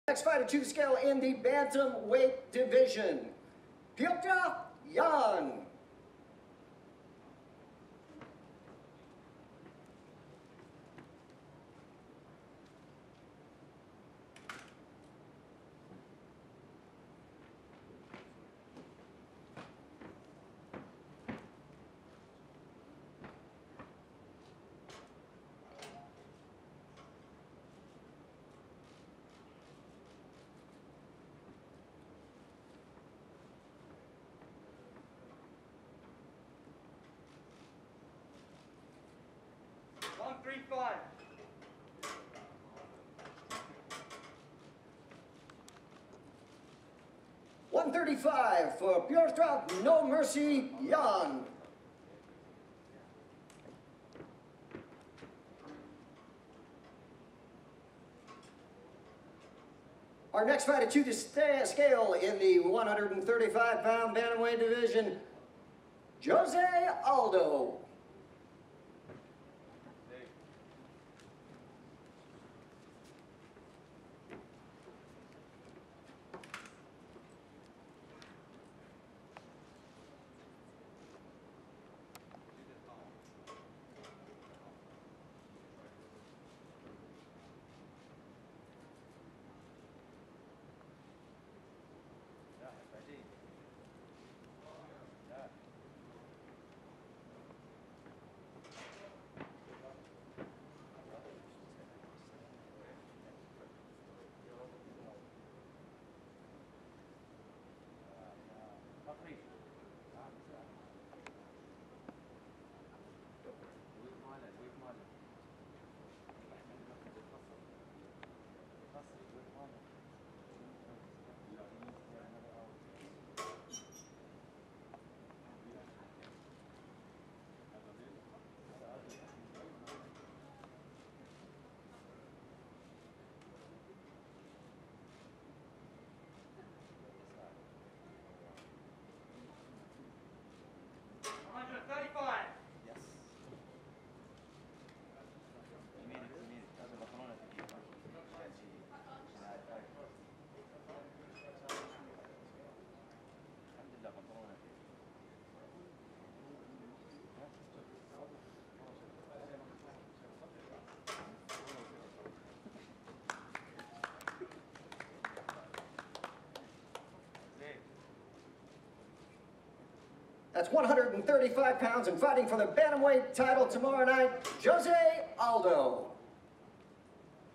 UFC 251 Weigh-Ins: Petr Yan, Jose Aldo Make Weight - MMA Fighting
At UFC 251 weigh-ins, Petr Yan and Jose Aldo both made bantamweight title weight Friday morning in Abu Dhabi.